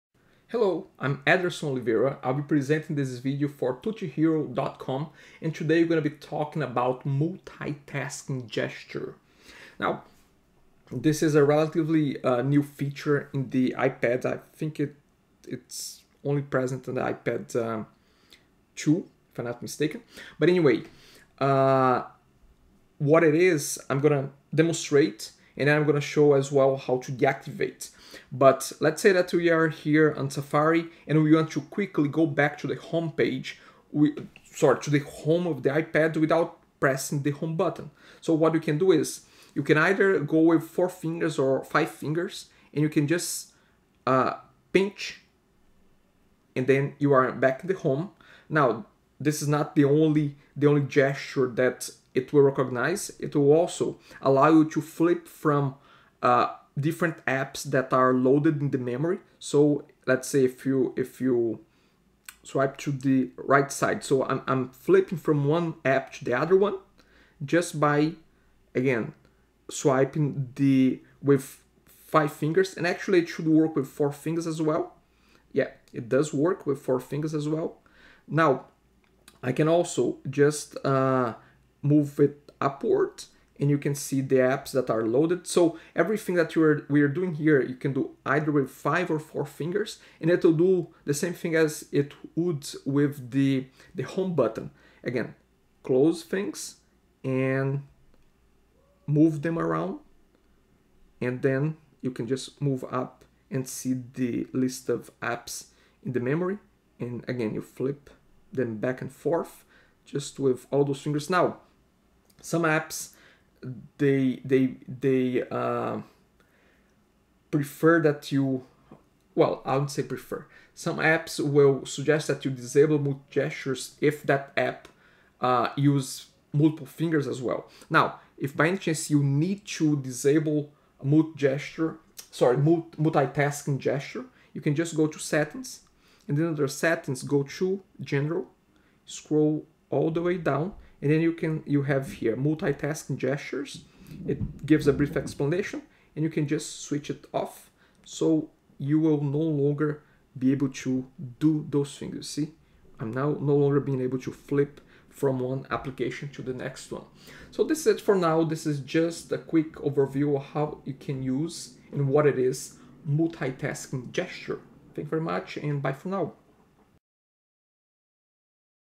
iPad Tutorial - What is Multitasking Gestures - v21
- Let's have a look at what Multitasking Gestures are on the iPad.